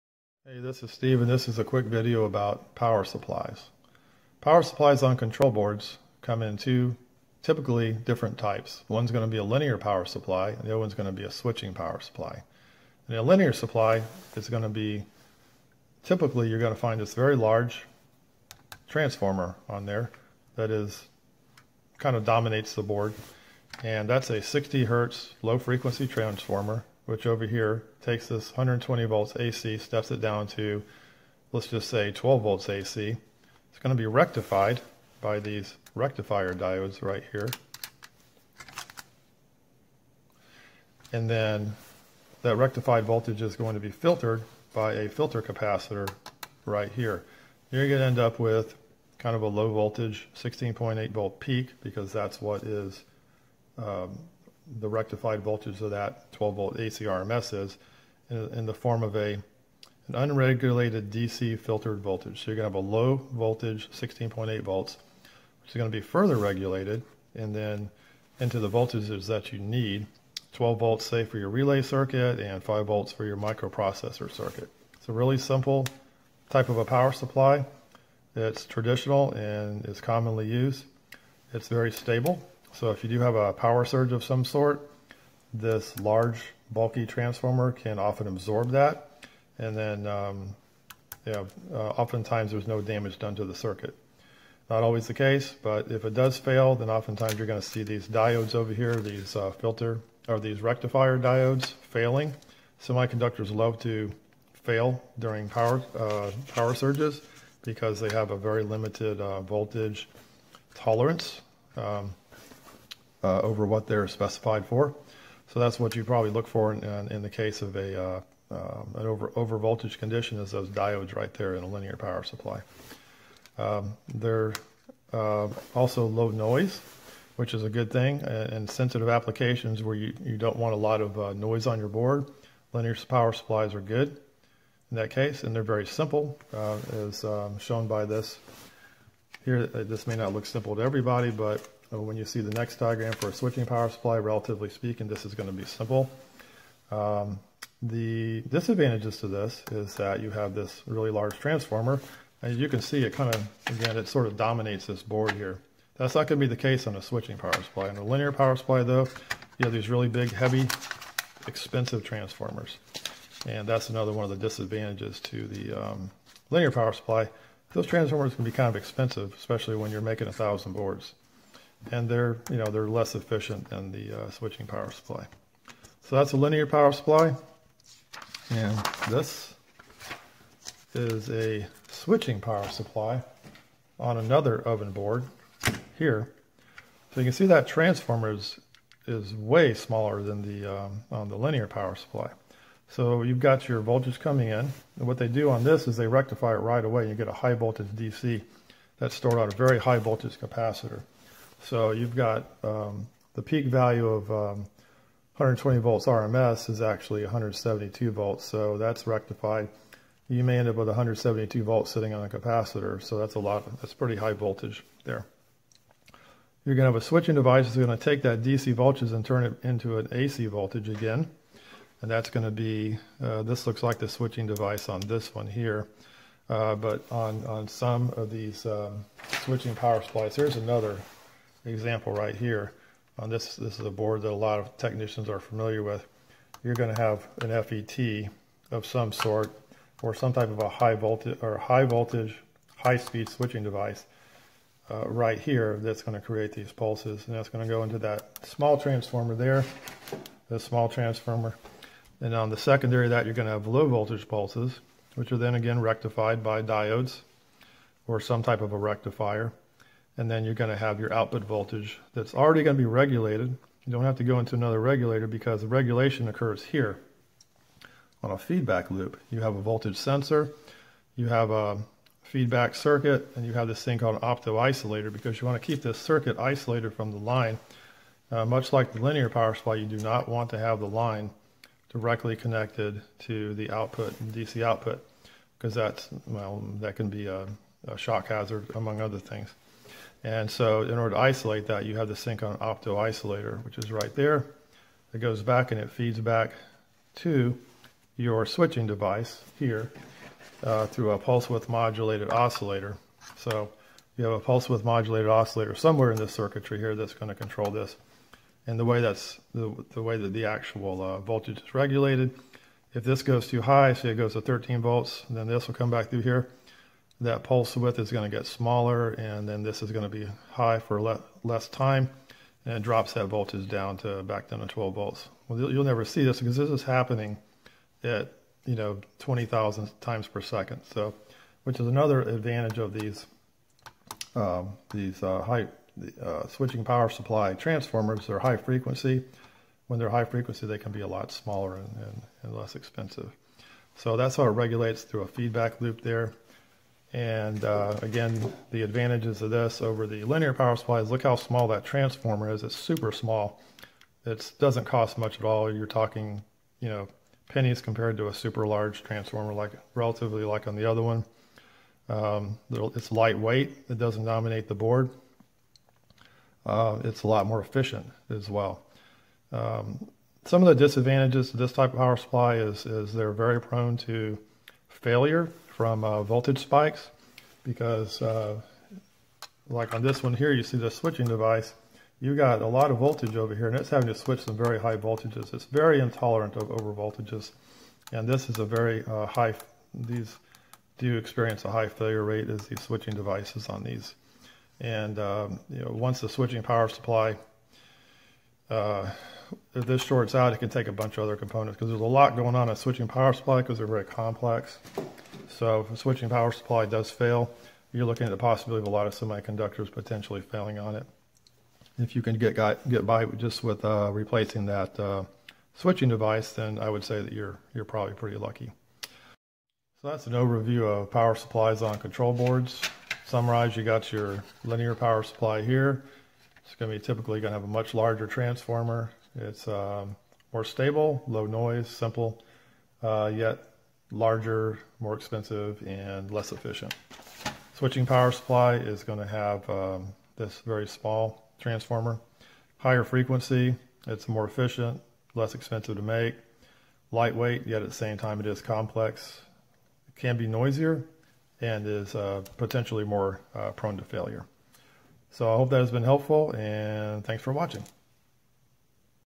#8 - How Power Supplies Work
Overview and block diagrams of Linear and Switching Power Supplies for Appliance Repair Technicians. Pros and cons of each type and common failures. Read the full article here: